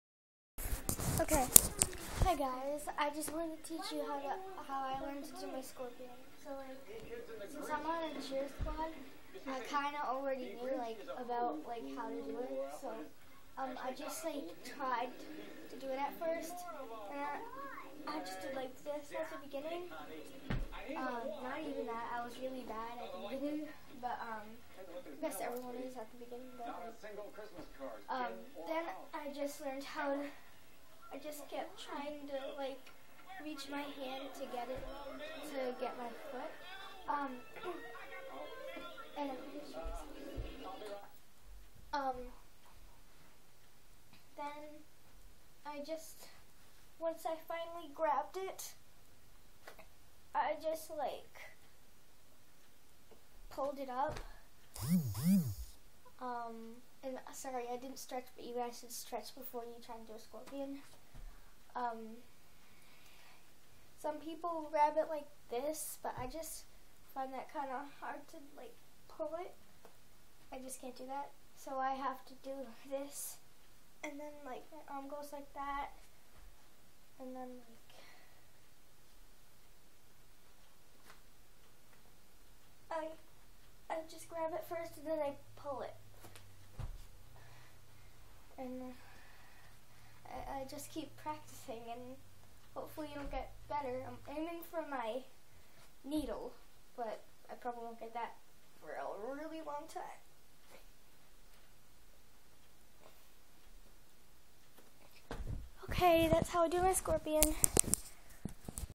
how i learned my scorpion.mp4
I hope you guys enjoyed. And before you try to do anything like this you should ALWAYS stretch before attempting something:-)